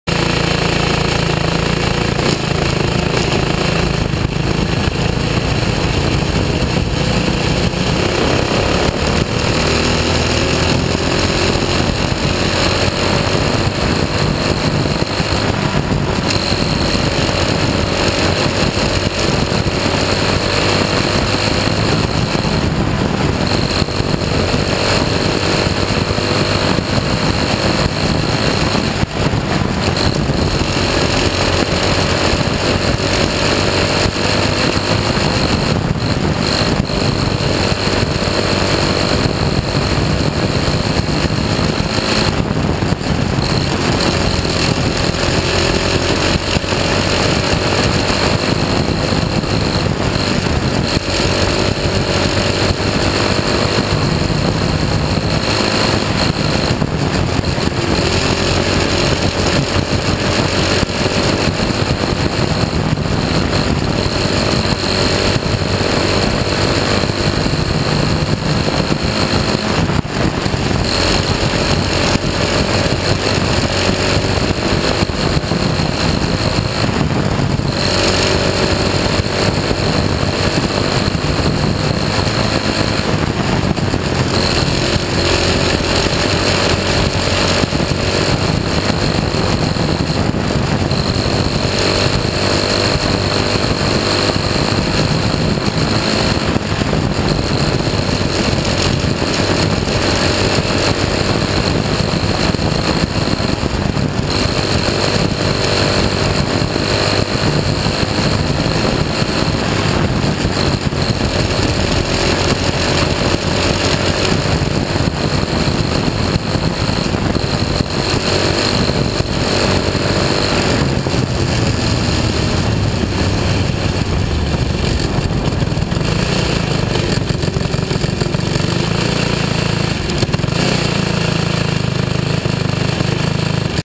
ZACH WIGAL I70/77 SPEEDWAY PRACTICE 4/30/16
After a short rain delay Zach went out without the green restrictor plate and got some unrestricted practice in on a very smooth racing surface